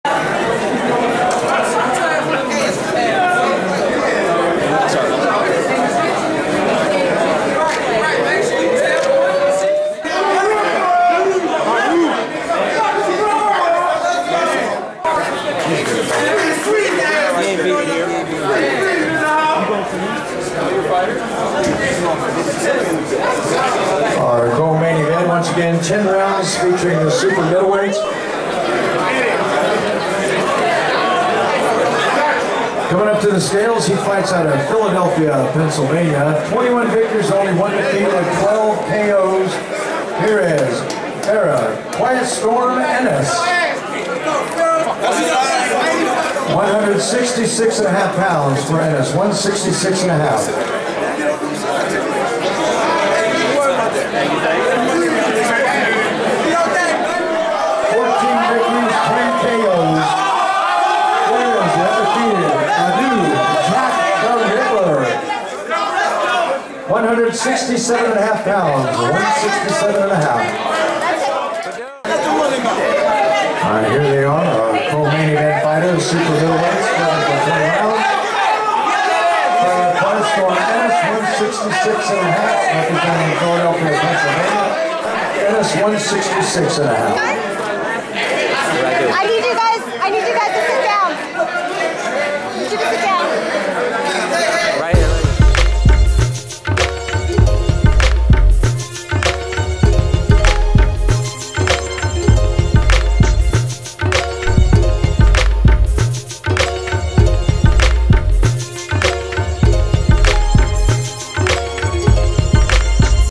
Badou Jack vs. Farah Ennis weigh-in coverage from the Hard Rock ahead of their Showtime battle
Chris Robinson was on hand inside of the Hard Rock Hotel and Casino in Las Vegas, Nevada as super middleweight prospects Badou Jack and Farah Ennis weighed in ahead of their Showtime-televised clash on Friday night.